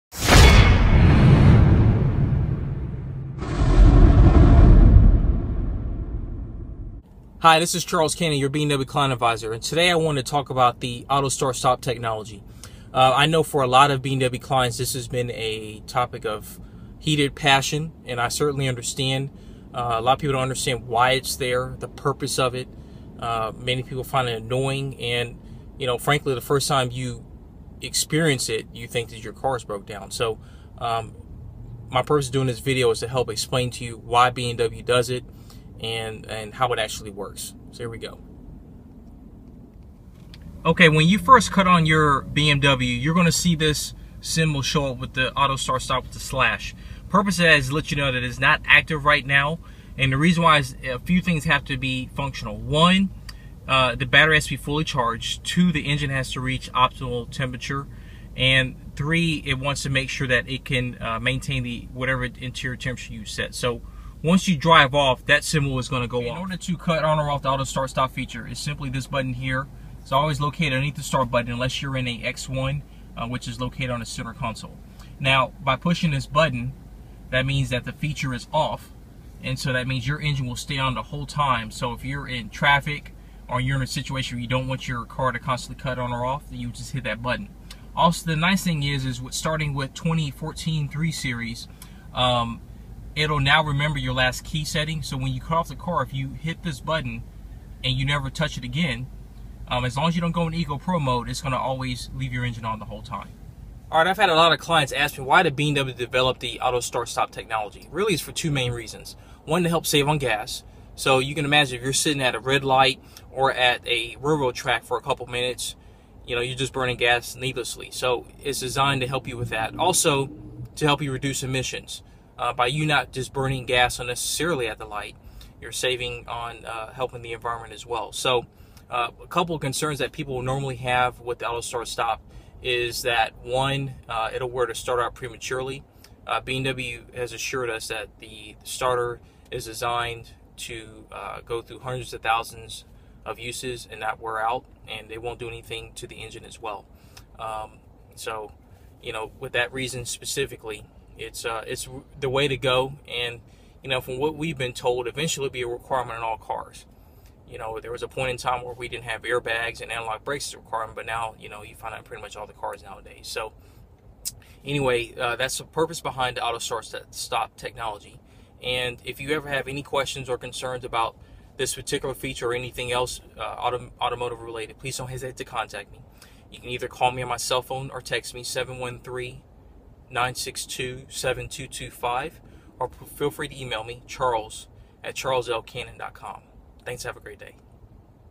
Why BMW has the auto start/stop feature Houston, Texas
For a lot of BMW clients, one of the most annoying features that they can’t understand is the auto start/stop feature.  My purpose of this video is to show you why BMW developed it.
It does 2 things:
1) reduces emissions by not having the engine running when not needed
2) helps to save you on gas.

Starting with 2014 models, you are also able to have it remember your last setting when you cut off your BMW, vs. having to turn the feature off every time you started the vehicle.